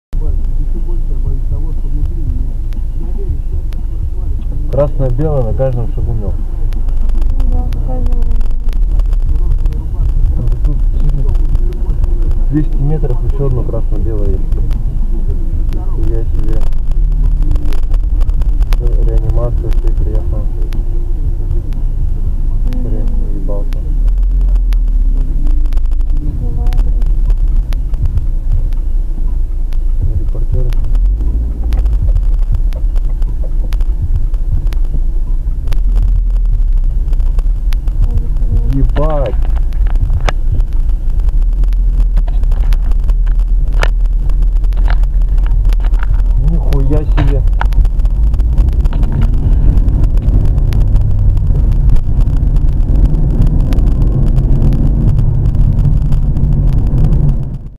Жесткое ДТП - 3 машины, Тюмень
Жесткое ДТП, Тюмень, 50 лет ВЛКСМ, после моста Мельникайте. 3 Машины. Потребовалась помощь реанимации.